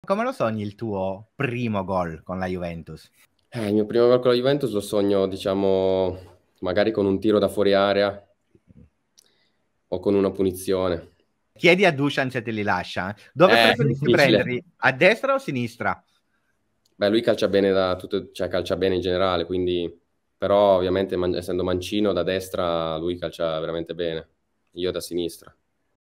DREAM DEBUT GOAL of HANS NICOLUSSI with JUVENTUS 🎯
Ciao Juventini of the world!
I had the immense pleasure to interview Hans Nicolussi Caviglia.
I asked him to describe his dream debut goal with Juventus.